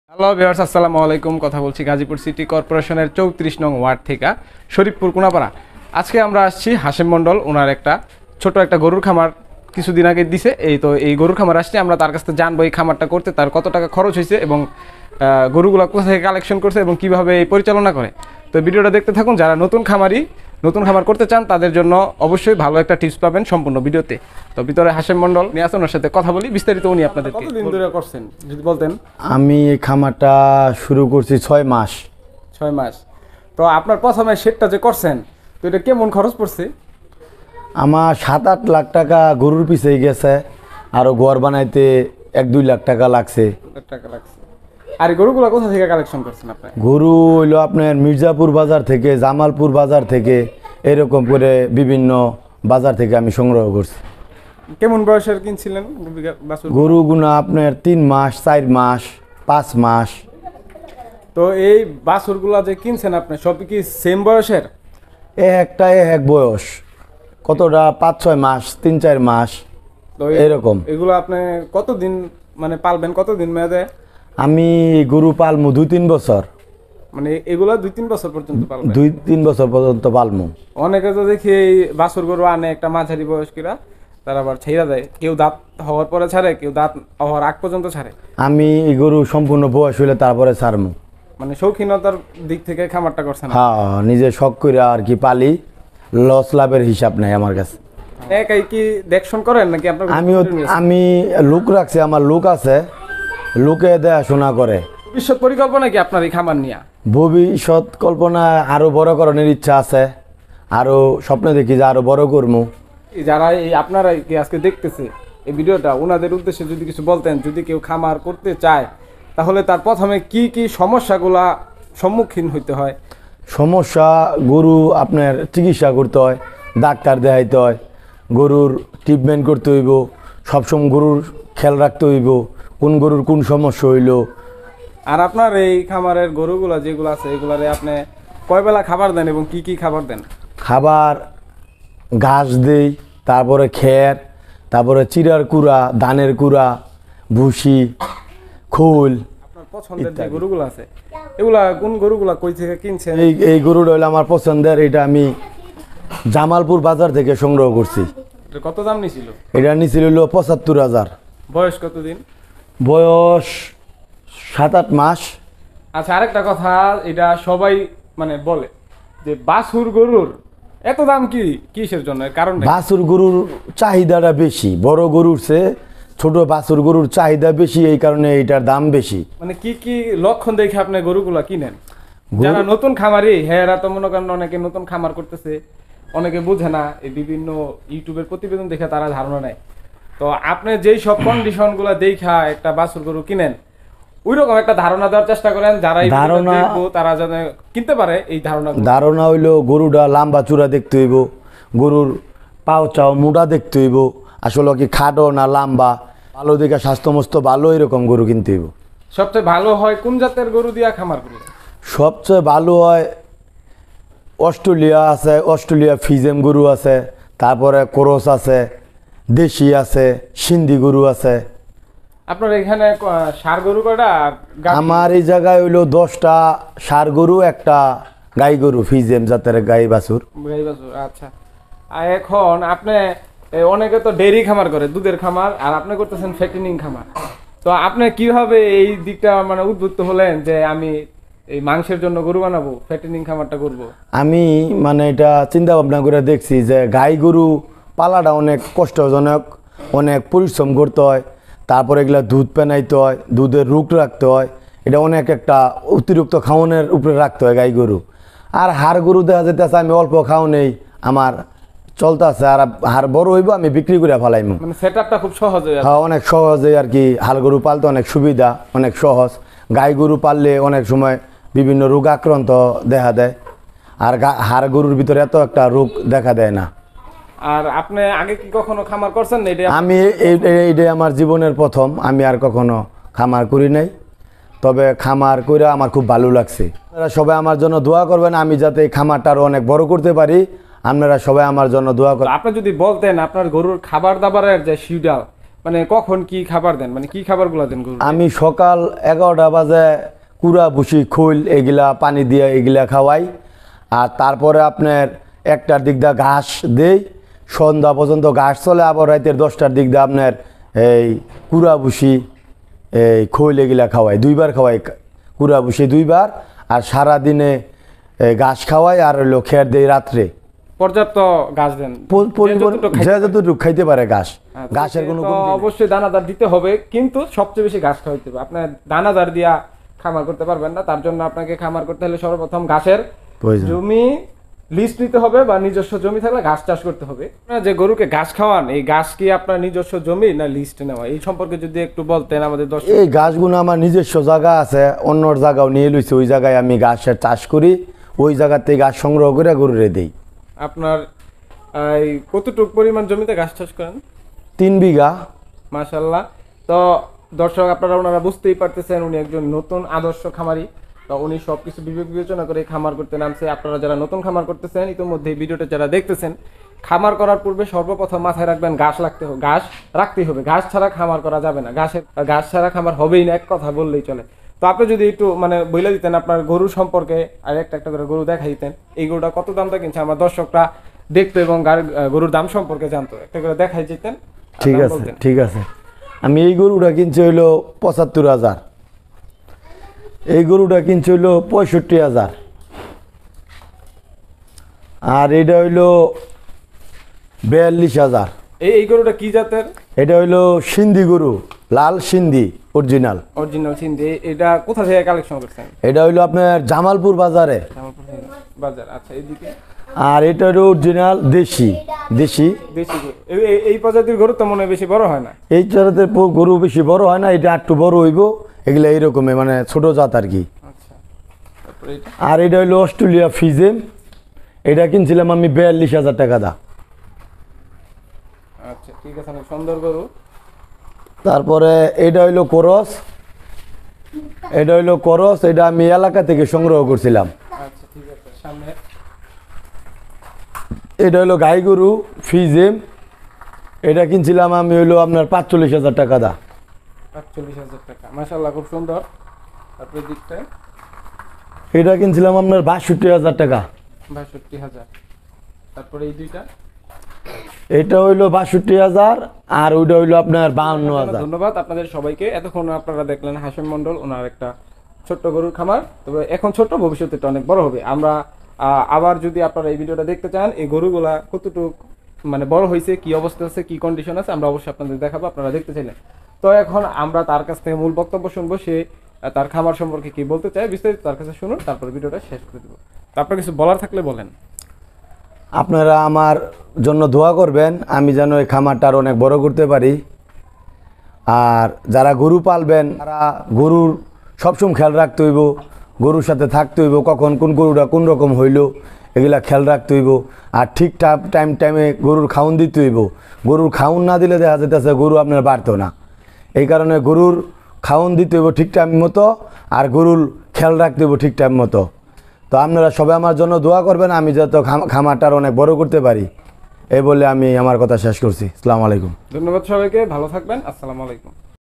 লাভবান হতে চাইলে প্রথমে গাভী নয় ষার দিয়ে শুরু হোক। আপনার শখের গরুর খামার Gazipur vlog
আসসালামুয়ালাইকুম গাজীপুর ব্লগে সবাইকে স্বাগতম আশা করছি সবাই ভালো আছেন। আজকের ভিডিওটি ধারণ করা হয়েছে শরীফপুর কোনাপাড়া 34 নং ওয়ার্ড গাজীপুর সিটি কর্পোরেশন। মোঃ হাশেম মন্ডল এর গরুর খামার সম্পূর্ণ ভিডিওটি দেখার জন্য ধন্যবাদ আপনাকে। আপনার খামারের প্রতিবেদন করাতে আমাদের এই নাম্বারে যোগাযোগ করতে পারেন। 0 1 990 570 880